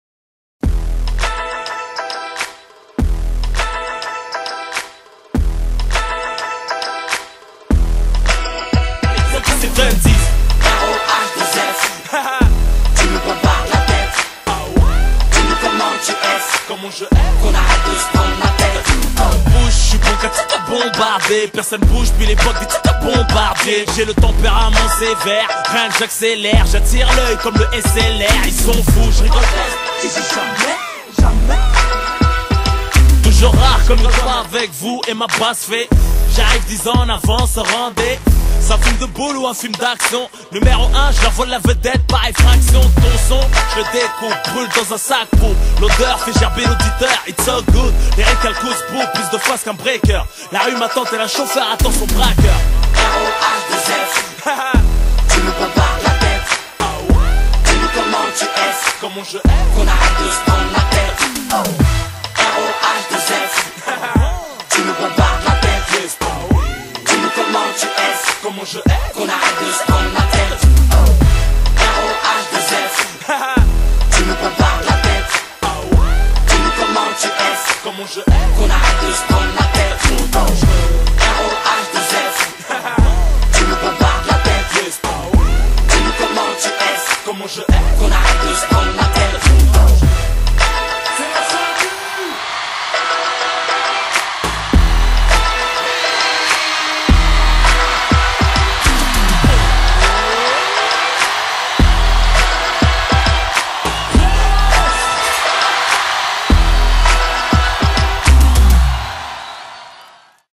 Skillz On The Ice - Hockey show - HD
| Hockey & Ice skating exhibition | Soutěžní odpoledne | Nízkoprahový klub COOLNA Znojmo | Exhibice HC Znojemští Orli | Skillz on the ice - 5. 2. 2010